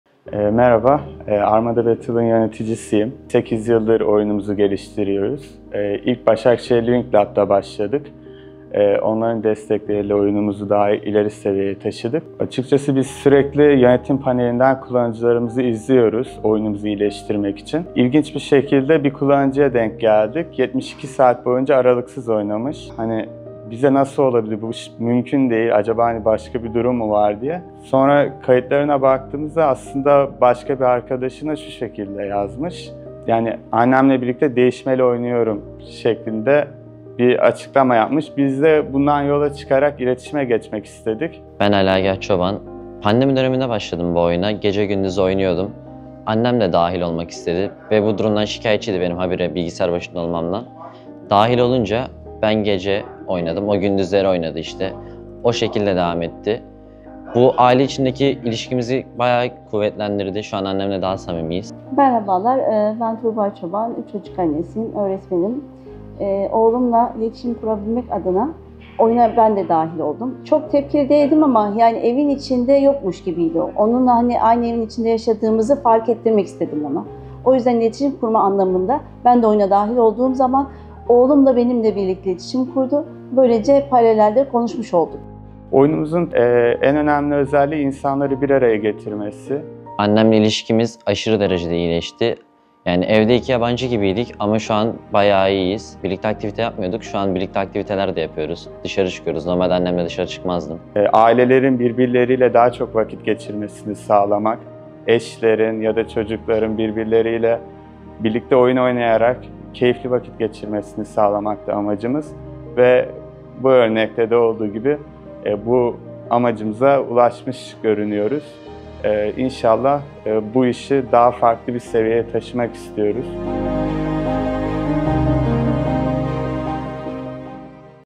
🎮 Aile bağlarını güçlendiren oyun başarısı!
Bir anne ve oğul, tam 72 saat boyunca oynadıkları oyunla sadece rekor kırmadılar, aynı zamanda aralarındaki bağı kuvvetlendiren bir yolculuğa çıktılar.